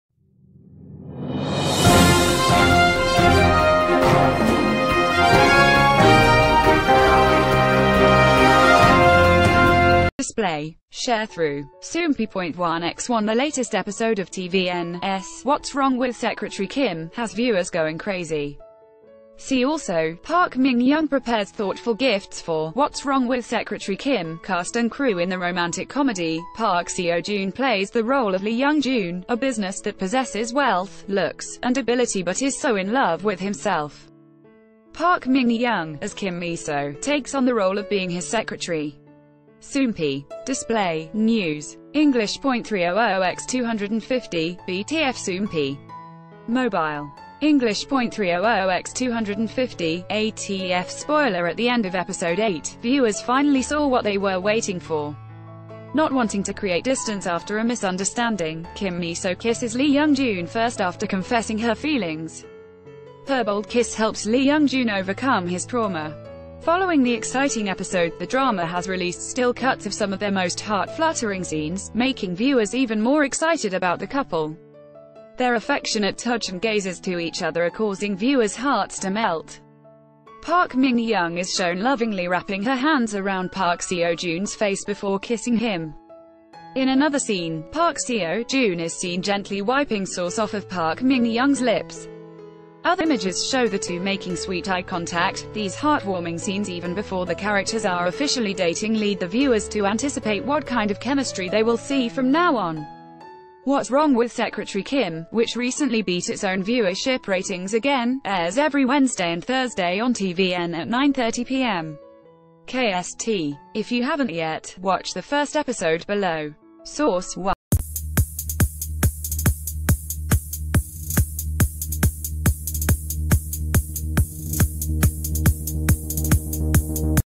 "What's Wrong With Secretary Kim" Reveals Romantic Images Of Park Seo Joon And Park Min Young
Display. Sharethrough. Soompi.1x1 The latest episode of tvN’s “What’s Wrong with Secretary Kim” has ...